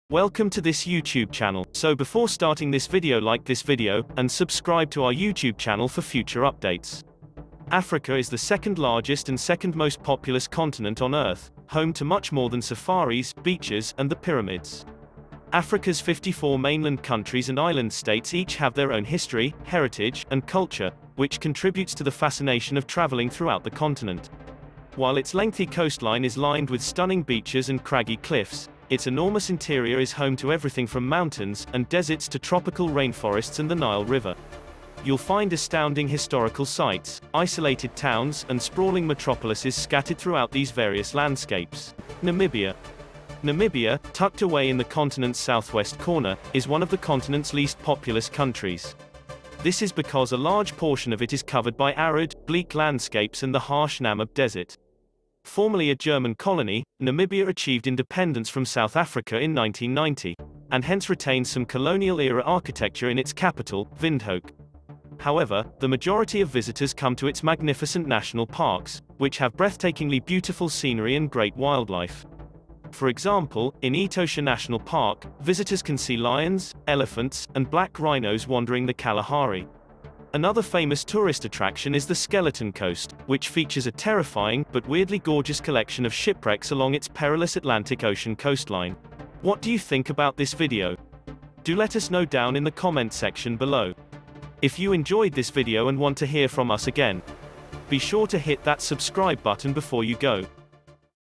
WHY NAMIBIA IS ONE OF THE BEST COUNTRY TO VISIT IN AFRICA
Namibia, tucked away in the continent's southwest corner, is one of the continent's least populous countries. This is because a large portion of it is covered by arid, bleak landscapes and the harsh Namib Desert. Formerly a German colony, Namibia achieved independence from South Africa in 1990, and hence retains some colonial-era architecture in its capital, Windhoek.
However, the majority of visitors come to its magnificent national parks, which have breathtakingly beautiful scenery and great wildlife. For example, in Etosha National Park, visitors can see lions, elephants, and black rhinos wandering the Kalahari. Another famous tourist attraction is the Skeleton Coast, which features a terrifying but weirdly gorgeous collection of shipwrecks along its perilous Atlantic Ocean coastline.